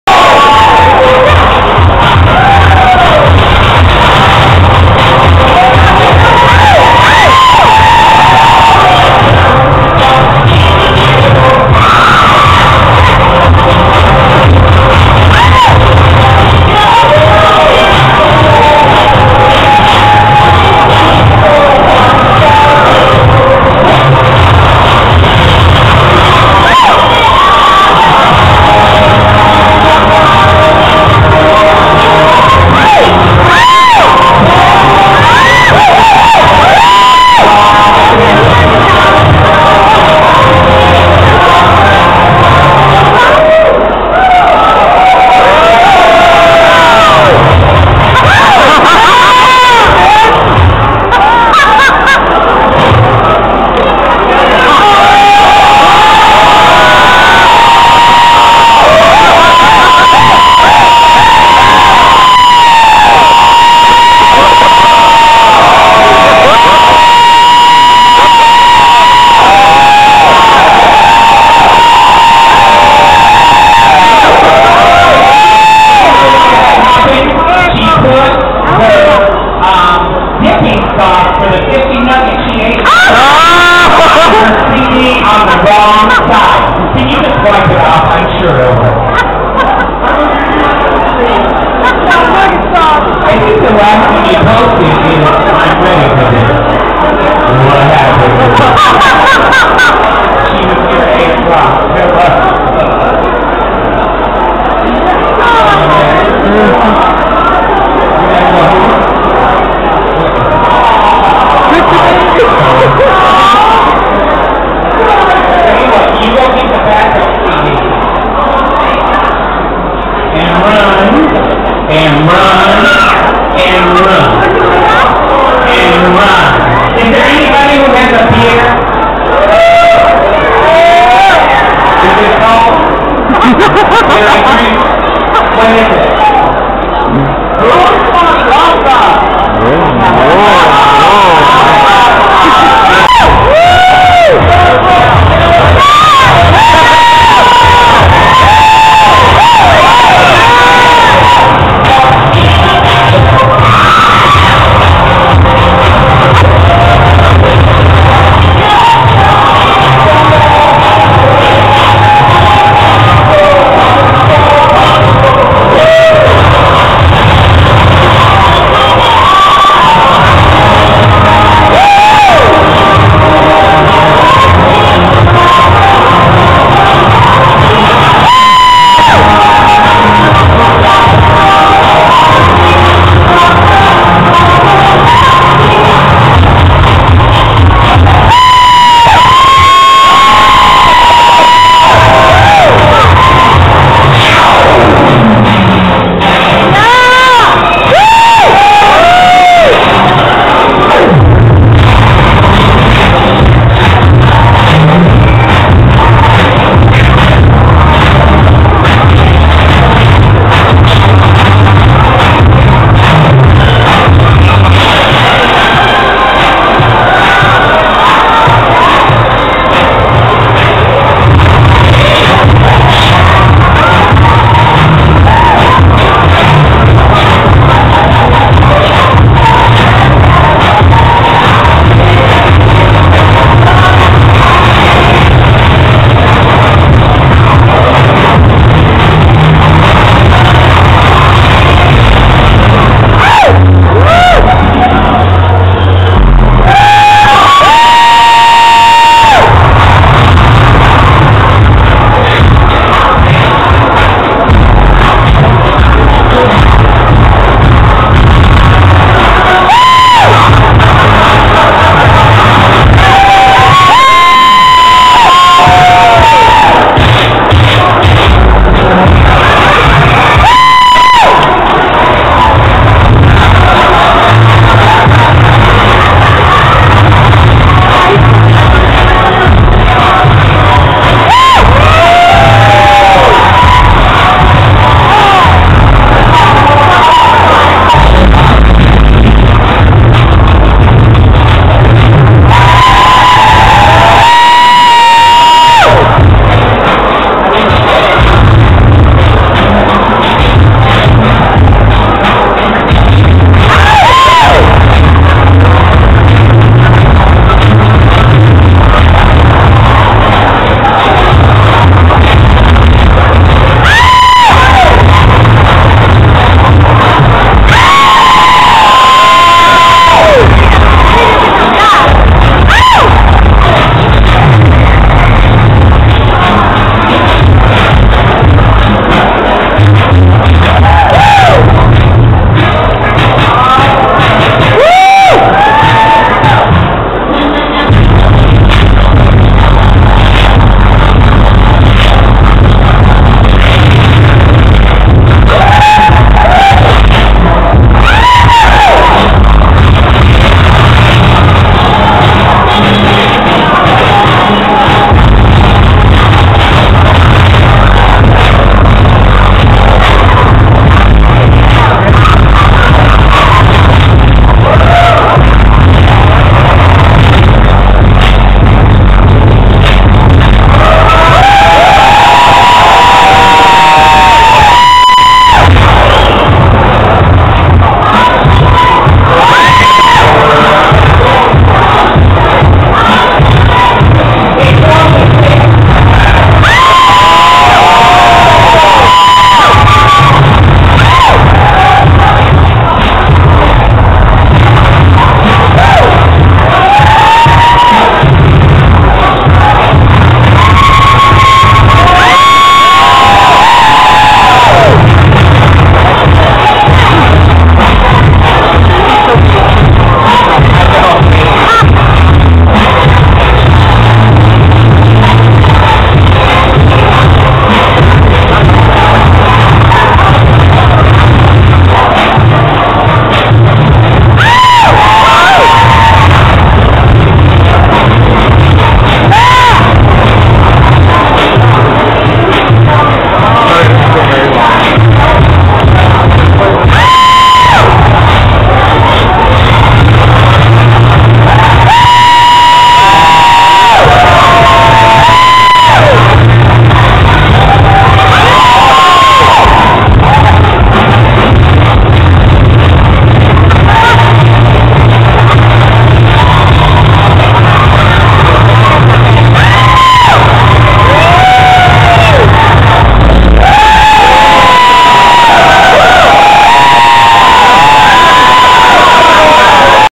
Azhia Li Kincade @ Space Coast Pride 2011 Part 1 & 2
I Performed at Space Coast Pride 2011..theres 2 parts bc part 1 wasnt a full show (the music cut off & i had to go get my back up CD) part 2 is the full show..part 1 is the reason why we r great entertainers..its bc no matter what happens in your performance YOU KEEP GOING and dancing :) sorry if the sound id grungy..my camera sucks lol :P